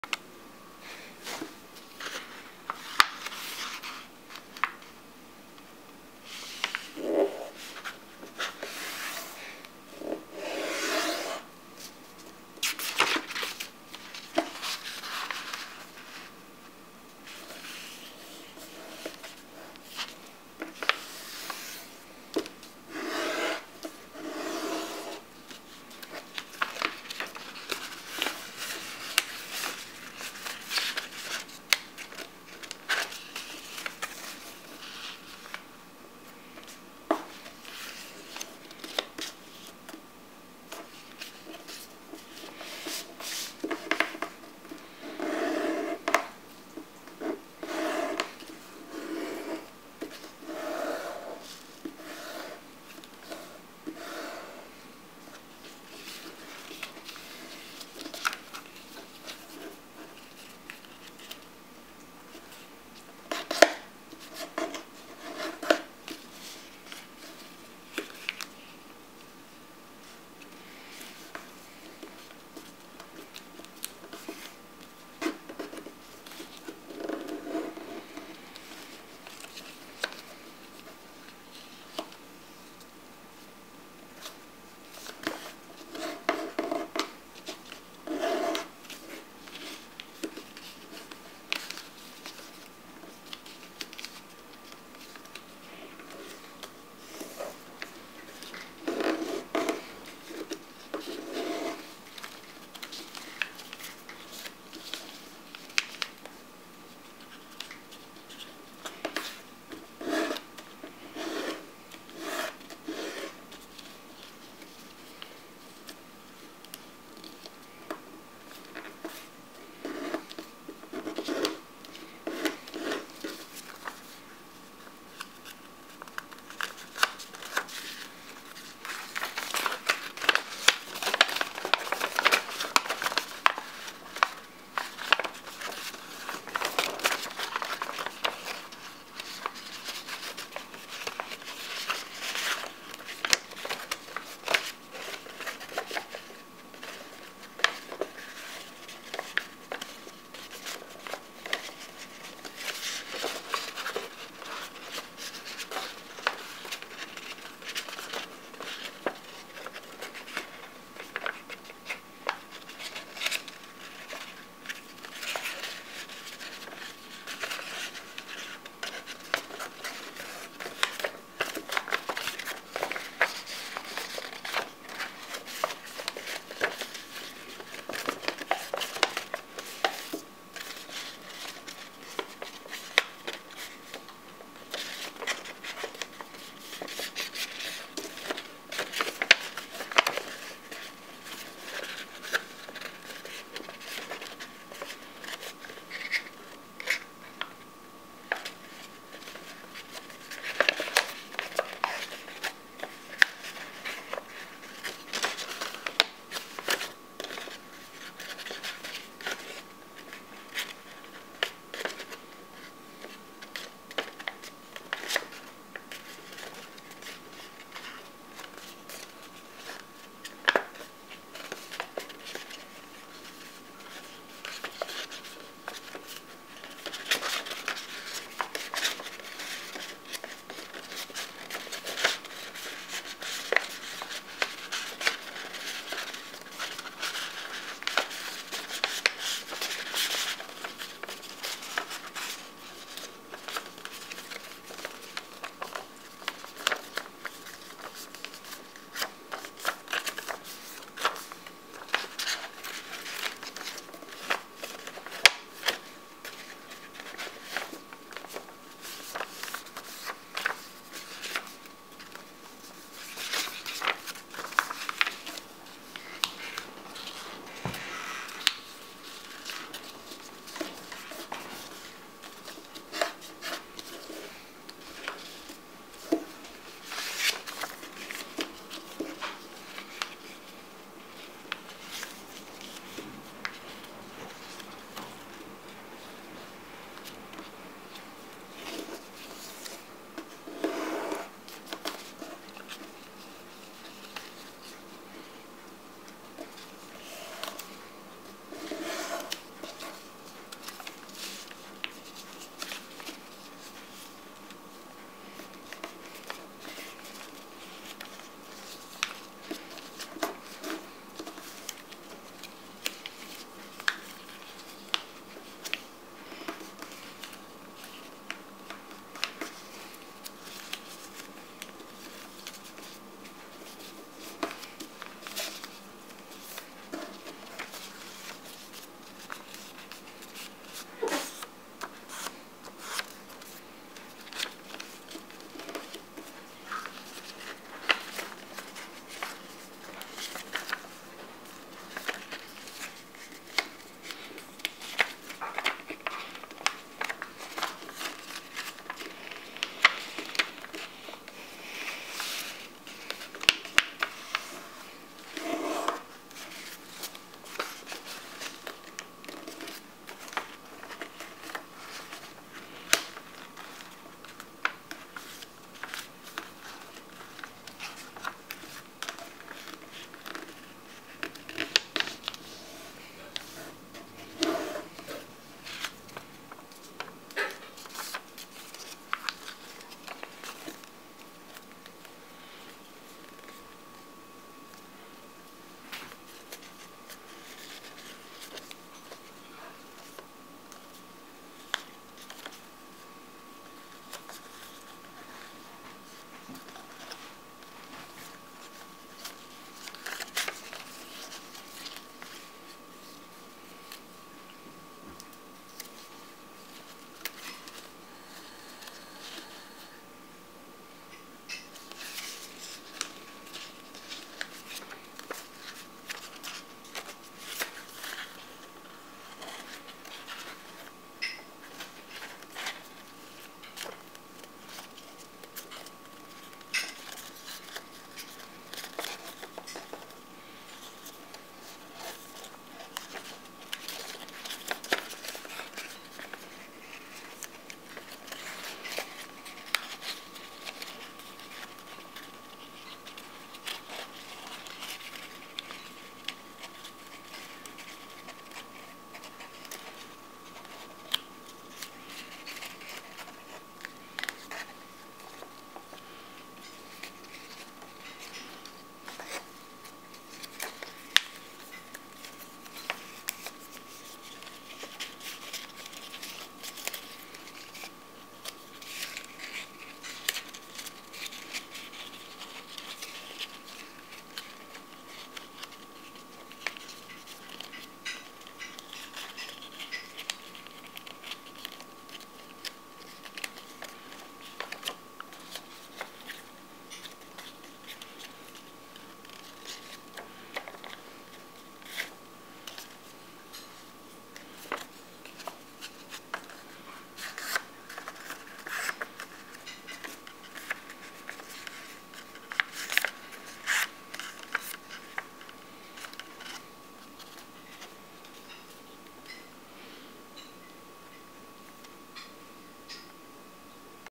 How to make a cool paper plane origami: instruction| Twin-Engine
Following this video you would be able to fold an amazing airplane. I will upload more models of planes in my youtube channel soon.